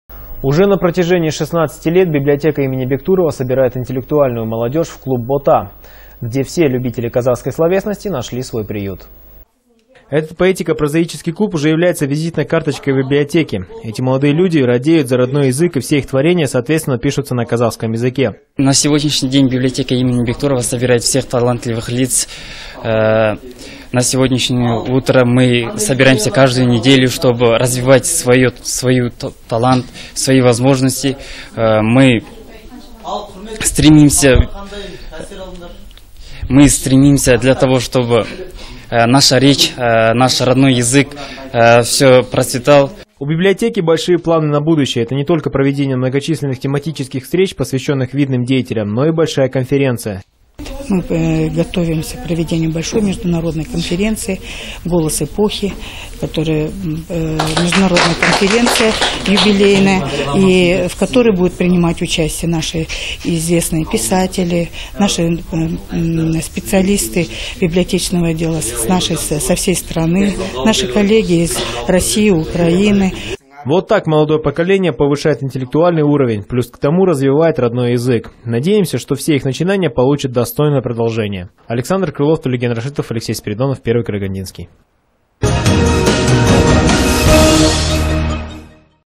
07_09_12_библиотека
Уже на протяжении 16ти лет библиотека имени Бектурова собирает интеллектуальную молодежь в клуб «Бота». Где все любители казахской словесности нашли свой приют.